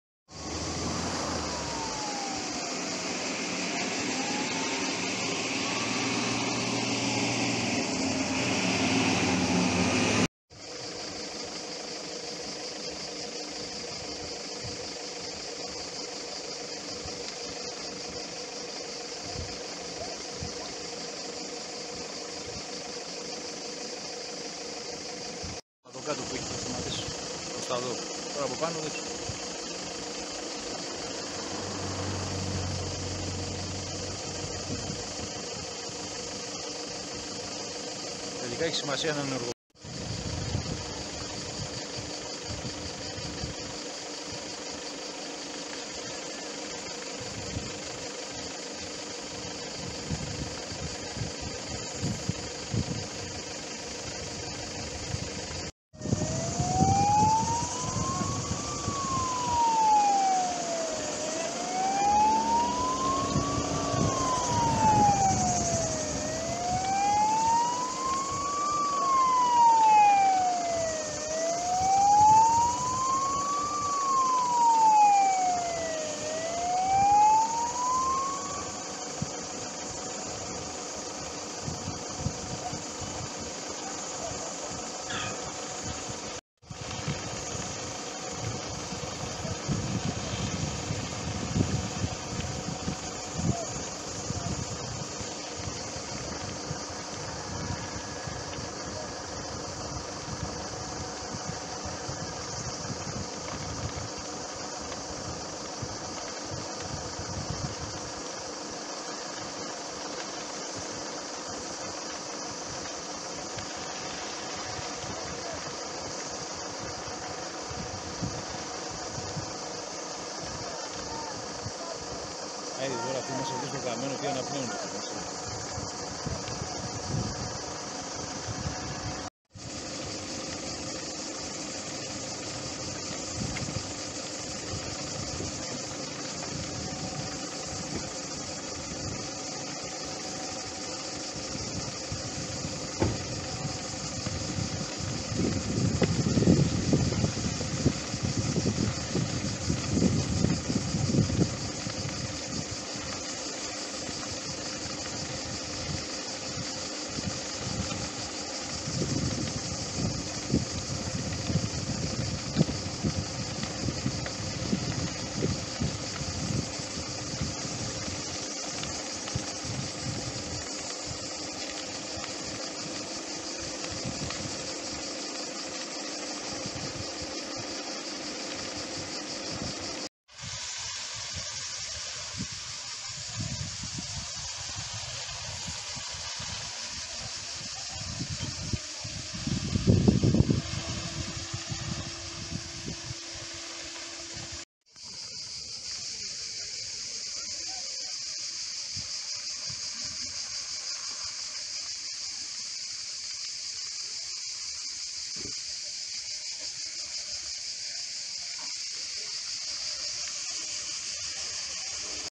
Φωτιά στο Ρέδι
Φωτιά ξέσπασε στη περιοχή Ρέδι του Αγίου Θωμά Βοιωτίας
Χάρη στην άμεση παρέμβαση της Πυροσβεστικής Υπηρεσίας με 6 οχήματα και 12 άντρες η φωτιά τέθηκε υπό έλεγχο και δεν απέιλησε κατοικίες.